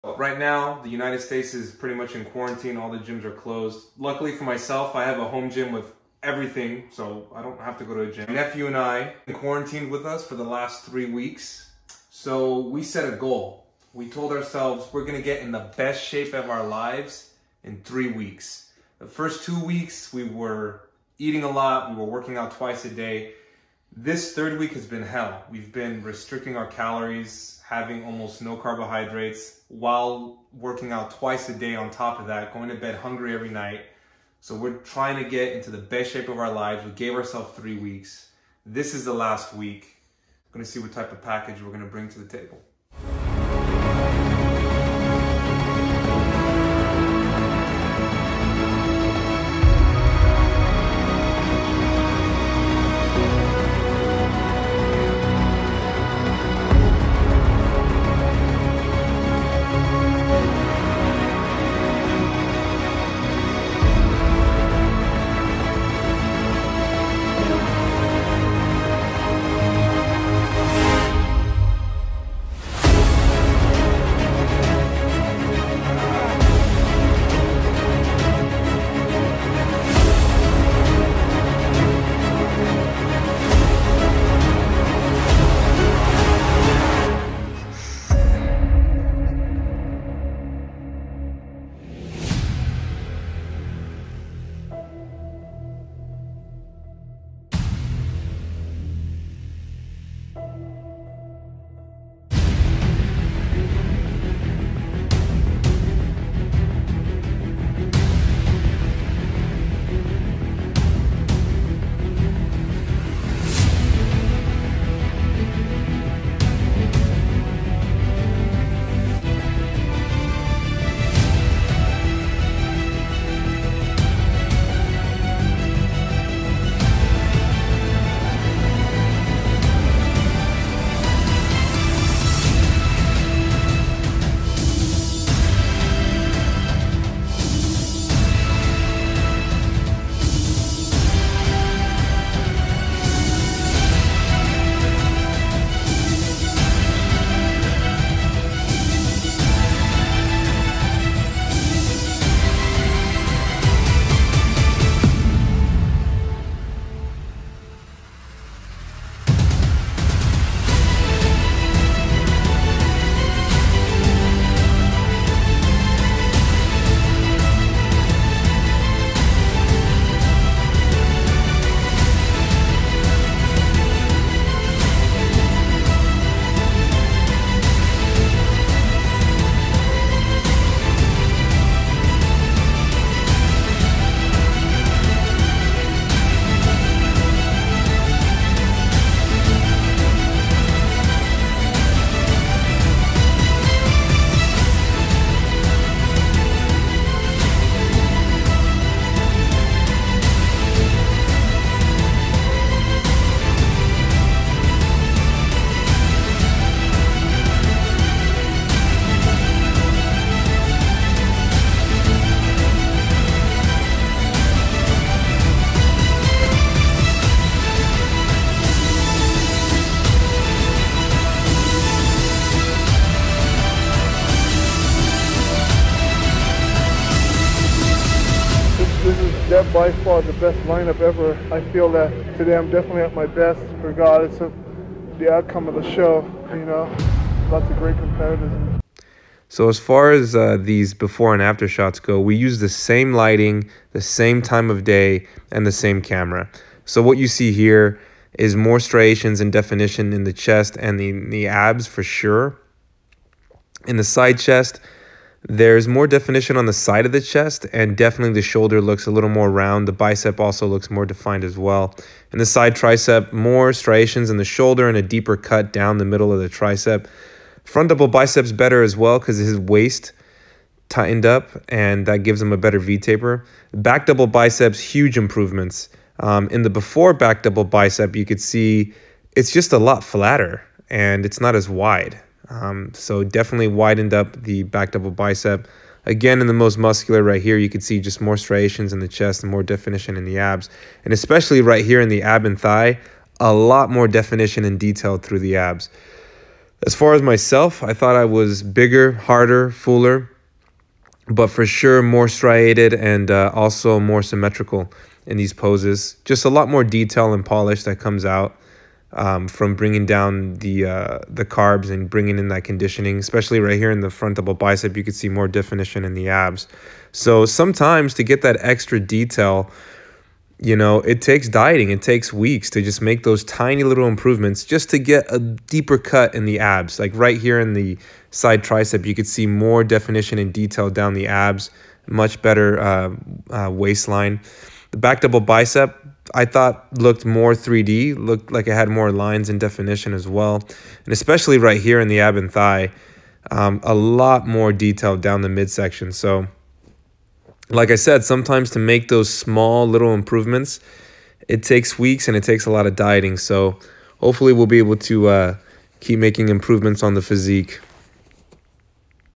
Shredded by the day vol 1
My nephew and I diet down to make improvements to our physiques.